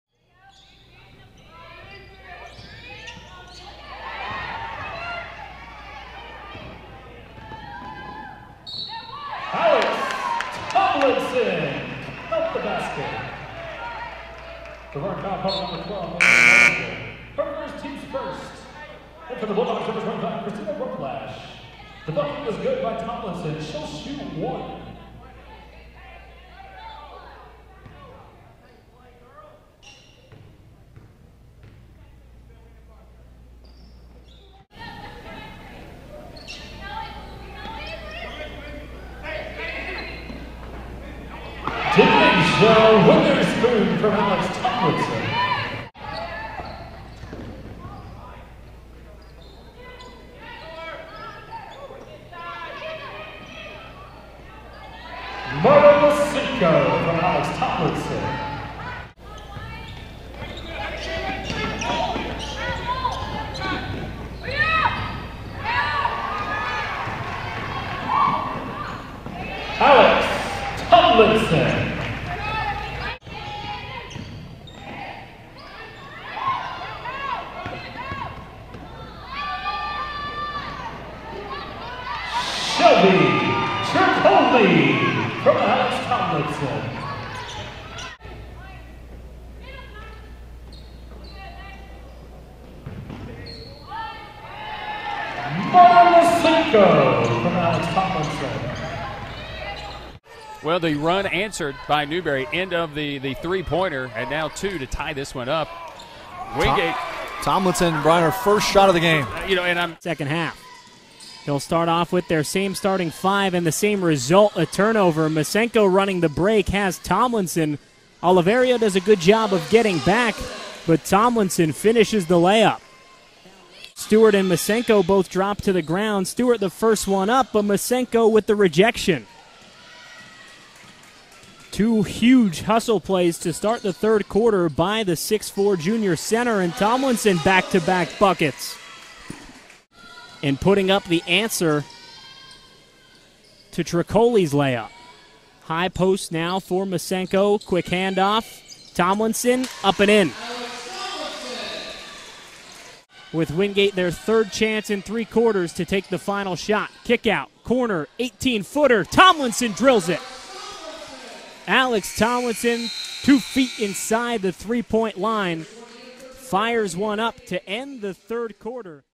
Alex Tomlinson - a few plays from her days at Wingate | Asst. Coach Radford Women's Basketball
Radford Highlanders Women's Basketball Assistant Coach, Alex Tomlinson, during her playing days at Wingate University.  Class of 2017.